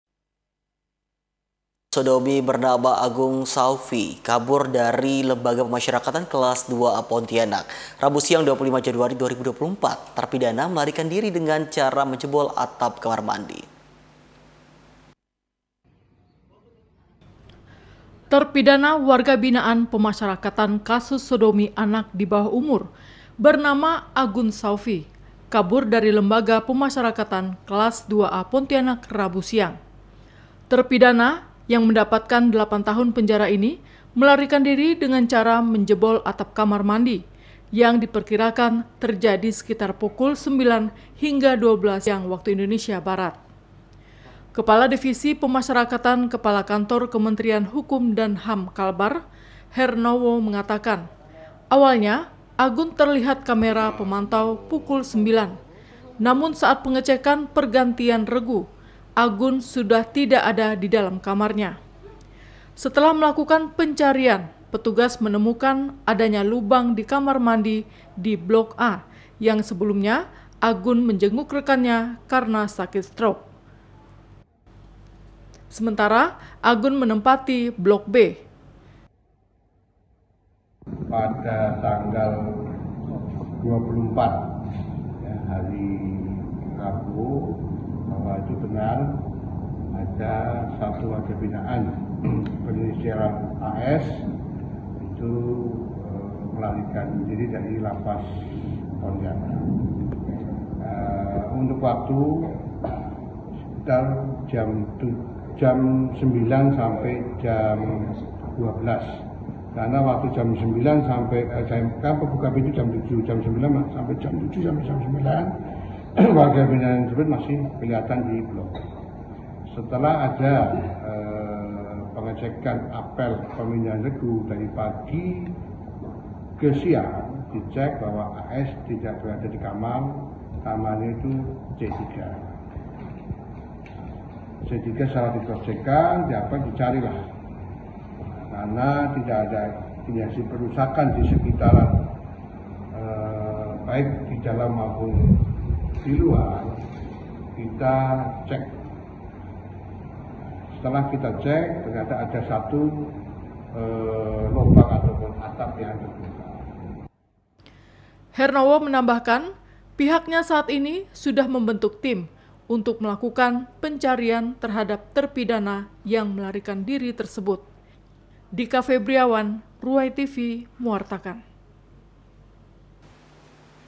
JEBOL ATAP KAMAR, NAPI KASUS SODOMI KABUR DARI LAPAS
erita Warta Ruai, 25 Januari 2024